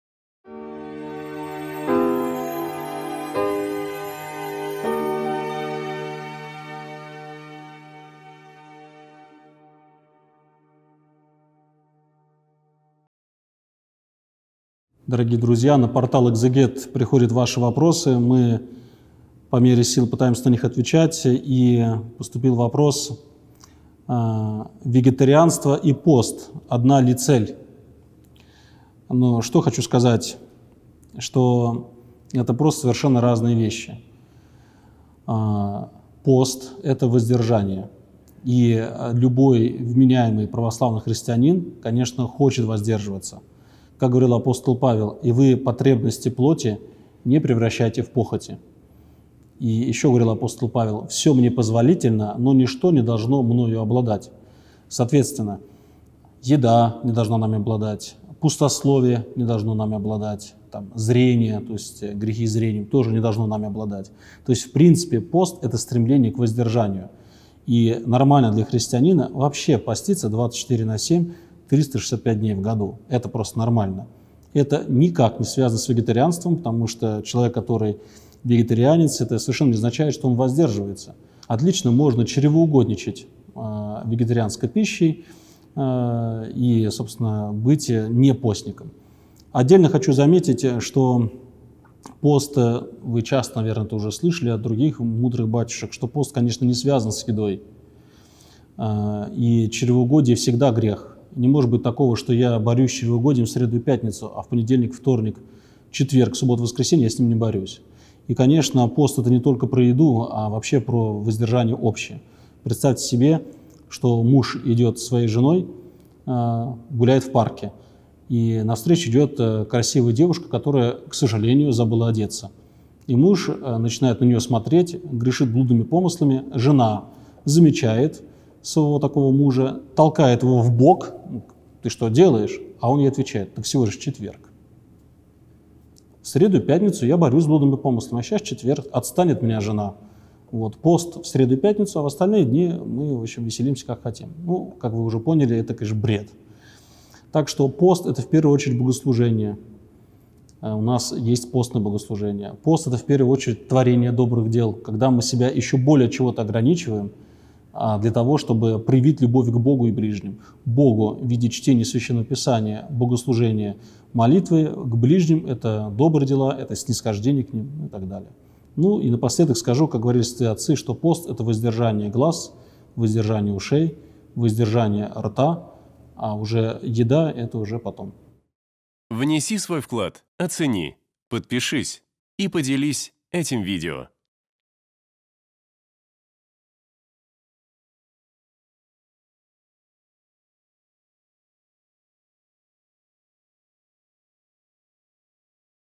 ВЕГЕТАРИАНСТВО И ПОСТ. Цель одна ? Священник Павел Островский
Команда Экзегет.

В сегодняшнем видео вместе со священником Павлом Островским отвечаем на вопрос:
"Вегетарианство или пост. Цель одна?"

🔸   Пост – это воздержание
🔸   Вегетарианство – образ жизни
🔸   Чревоугодие – грех
🔸   Проявление любви к Богу и к ближнему

📜 Читать Послание к Римлянам главу 13, стих 14 (с толкованиями)